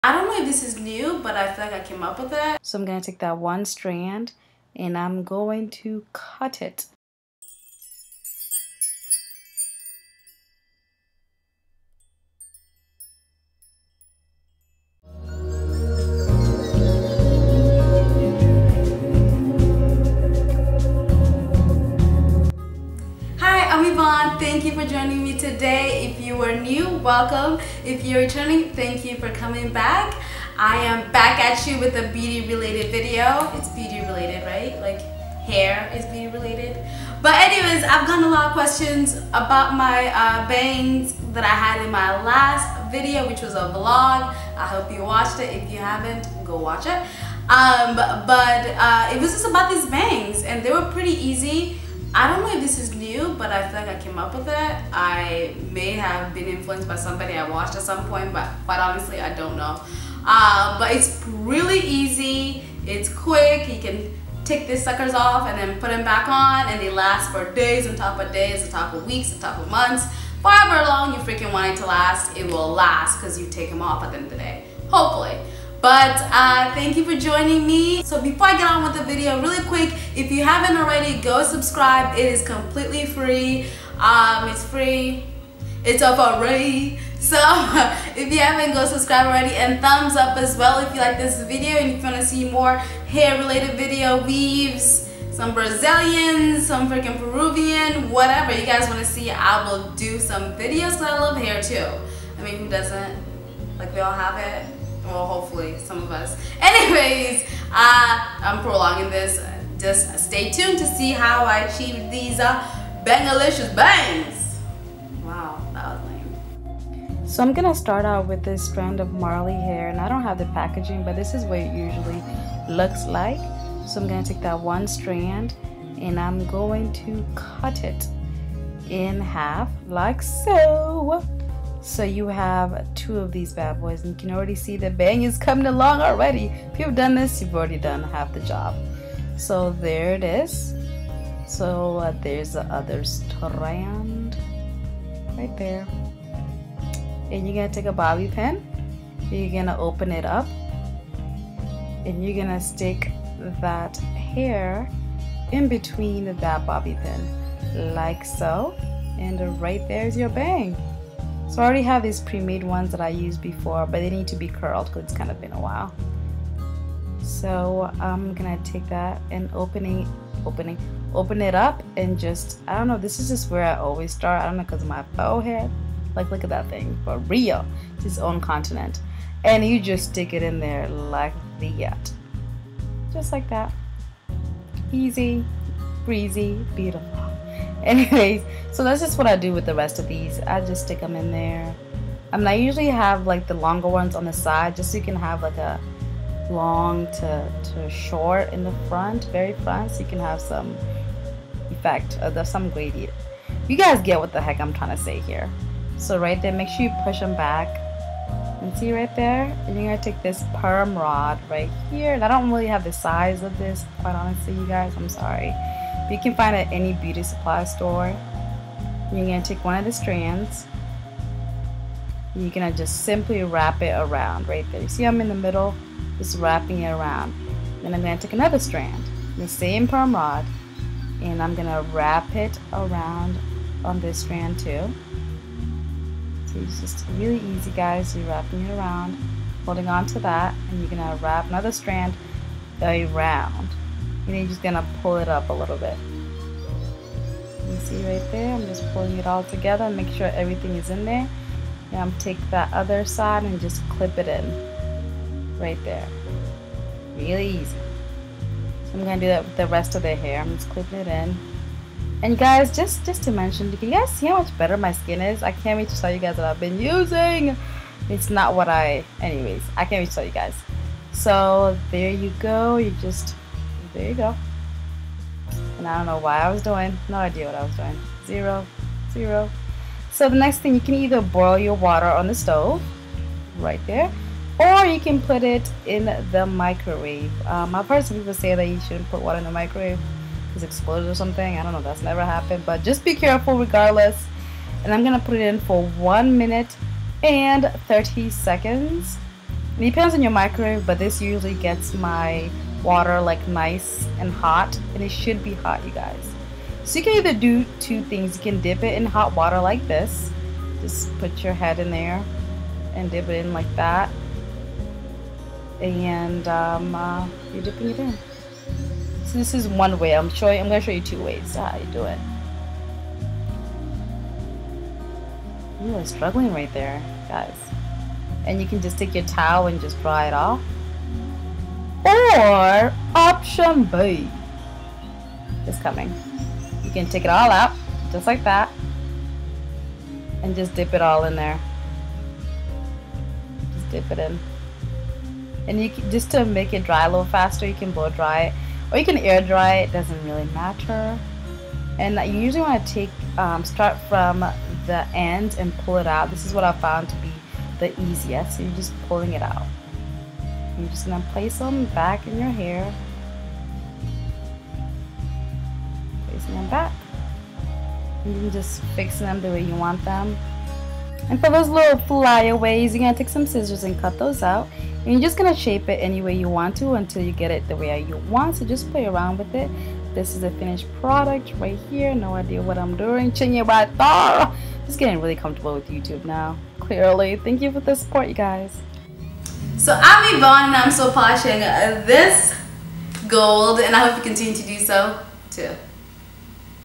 HOW TO: FAUX BANGS WITH MARLEY HAIR USING PERM RODS
For those naturalistas who want a quick hair change for a date, event, or just a different look overall these removable bangs are perfect! I hope you guys enjoy the video!